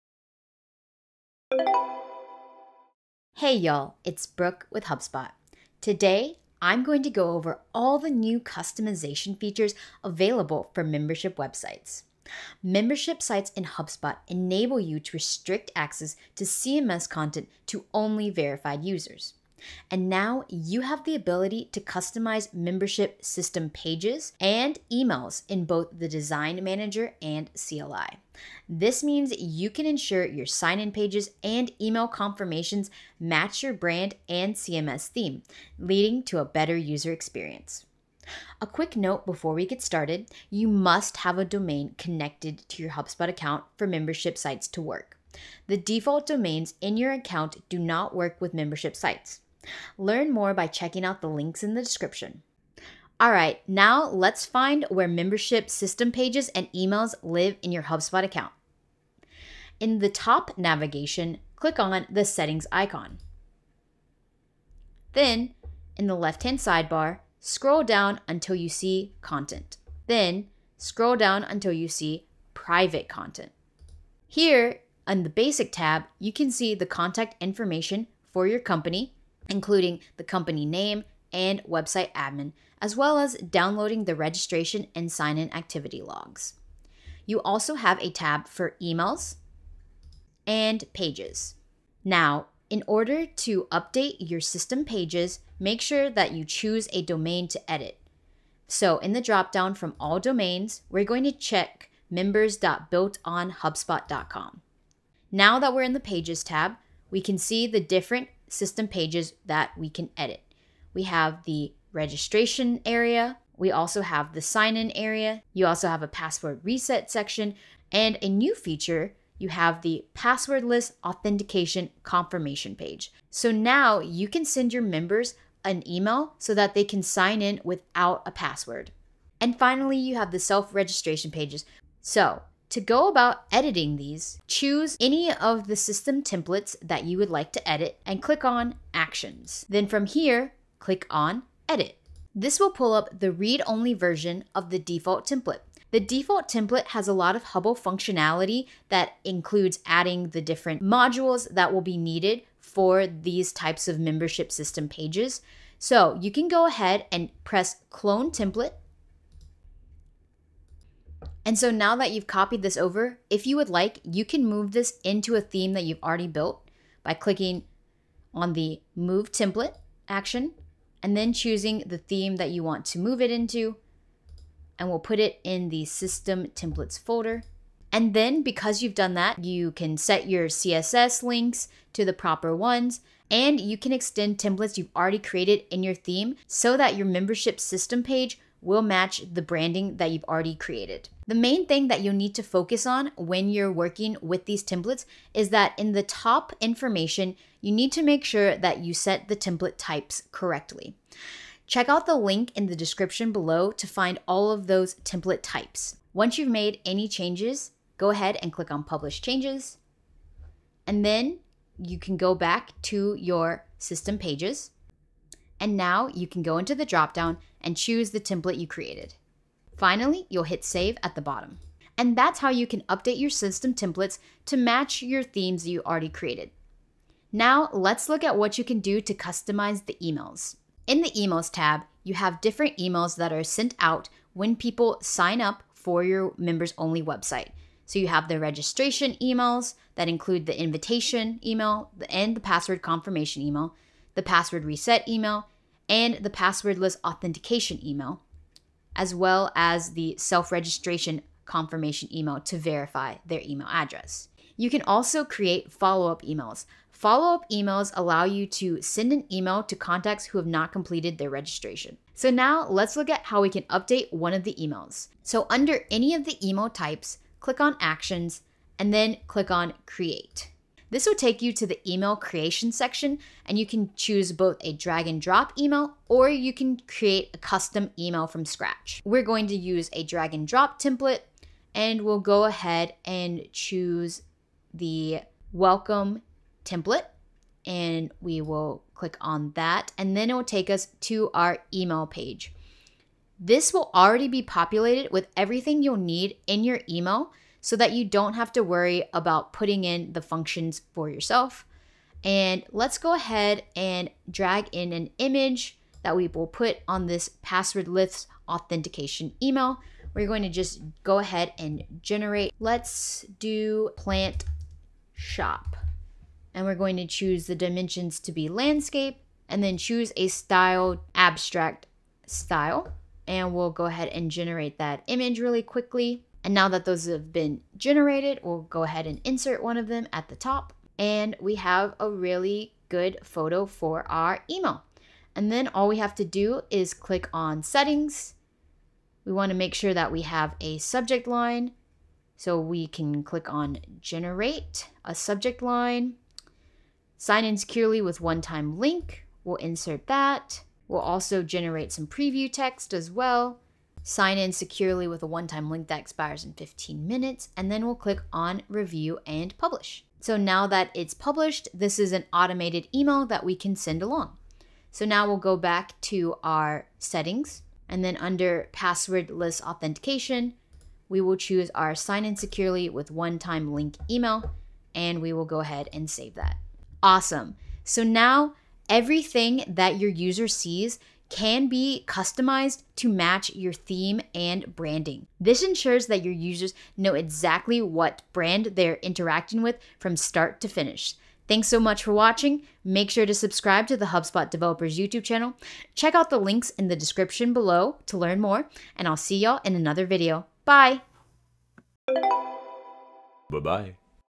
Customize Your Membership System Pages and Emails
Learn how to enhance your HubSpot membership sites with customizable system pages and emails. Membership system pages might get overlooked, but having them not adhere to your brand can lead to poor user experience. Now, you can customize these system pages to fit in with your theme while making use of the pre-built HubL modules. Also, you can now make use of the email designer to update your membership emails to match your brand. Ensure every touchpoint in a user’s membership journey is on-theme.